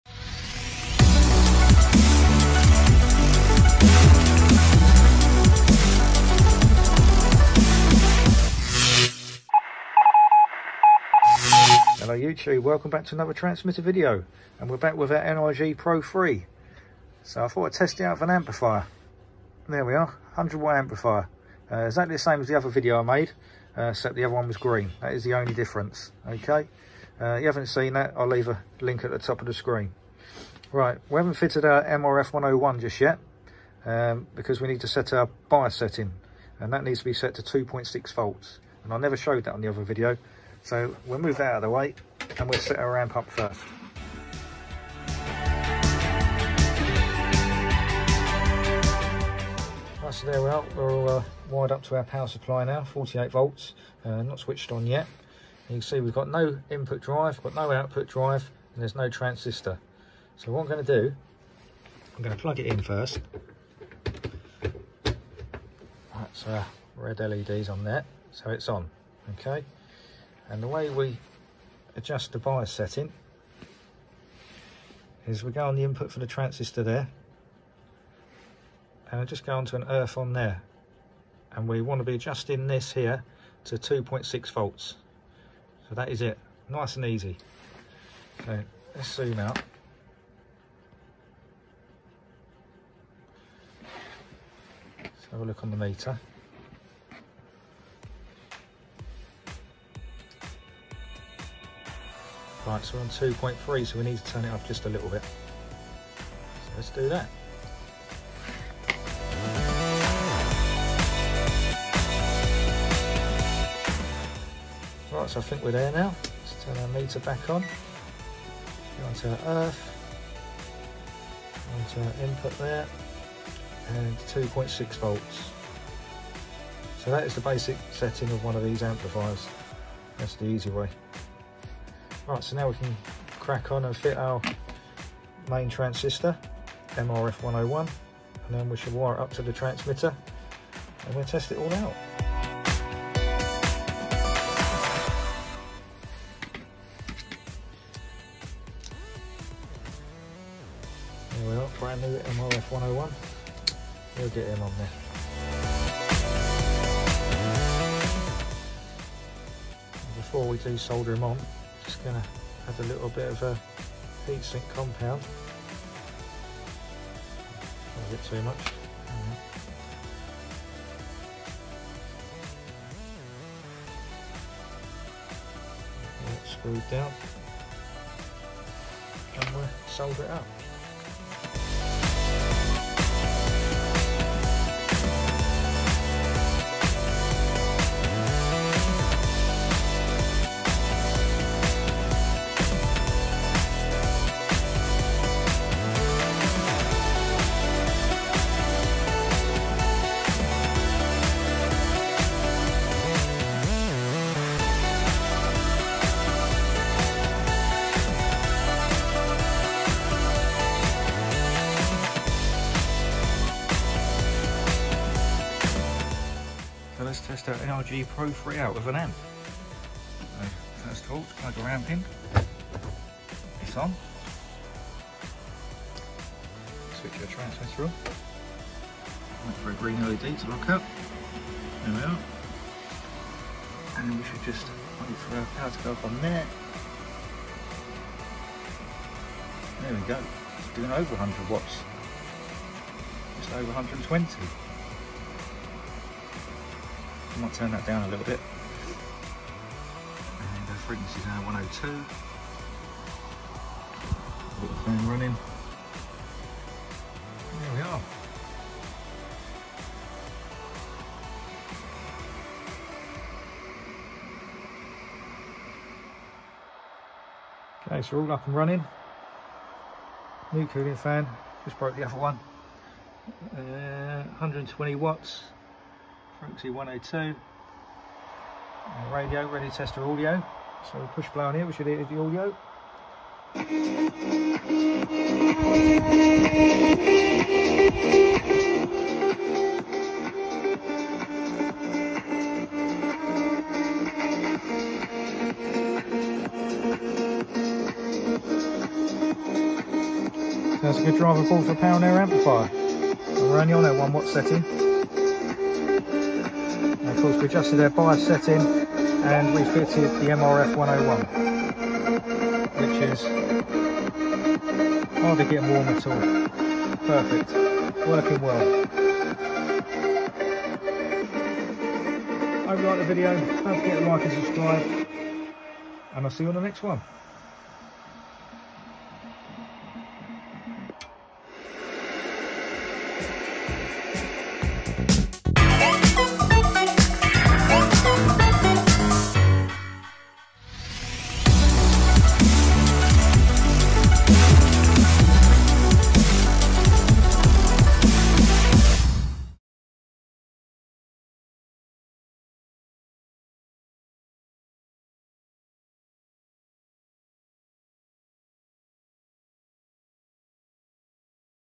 120Watt VHF Amp & Adjustment of BIAS - 87-108 Mhz -
Just for a bit of fun here is a little follow on video from my 100 watt amp build. I never showed how to set up the amp on the original video... so here it is and also testing the NRG PLL PRO 3 with an amp. The TX is one watt output/Drive.

Aron102